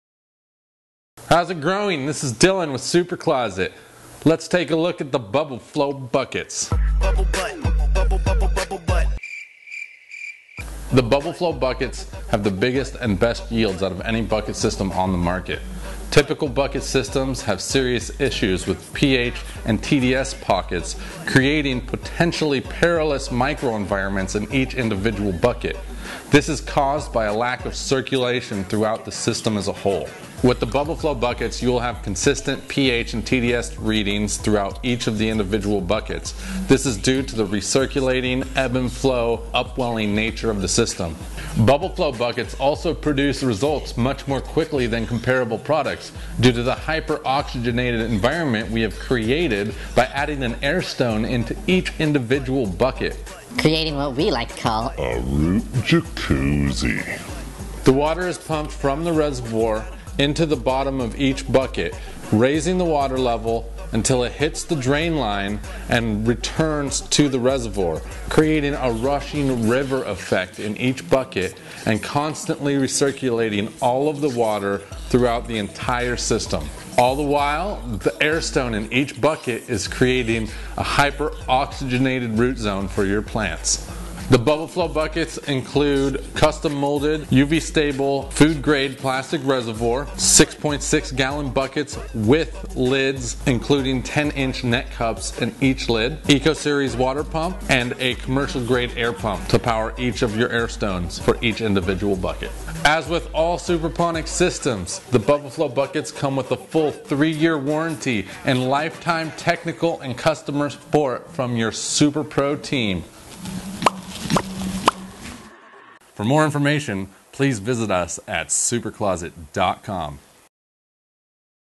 SuperCloset Review | Bubble Flow Buckets | Best Hydroponics System
SuperCloset's Bubble Flow Buckets grow the biggest and best yields of any bubble buckets system on the market, because they combine the technology of bubble buckets and ebb n’  flow buckets into one fully automated, recirculating hydroponic grow system.

Typical bubble buckets designs have constant issues with pH and TDS pockets creating potentially perilous micro environments in each bucket. This is due to a lack of circulation throughout the system as a whole. The Bubble Flow Buckets all have the exact same readings due to the addition of the re-circulating ebb n’ flow/up-welling feature. Bubble Flow Buckets therefore have consistent pH and TDS readings and even nutrient distribution throughout the entire system.

SuperCloset's Bubble Flow Buckets also produce results much more quickly than comparable products due to the hyper-oxygenated environment we have created in each individual bucket. It is a regular root Jacuzzi for each plant. Your plants will be so pleased with their new home that they will most definitely reward you for your kindness and good decision making.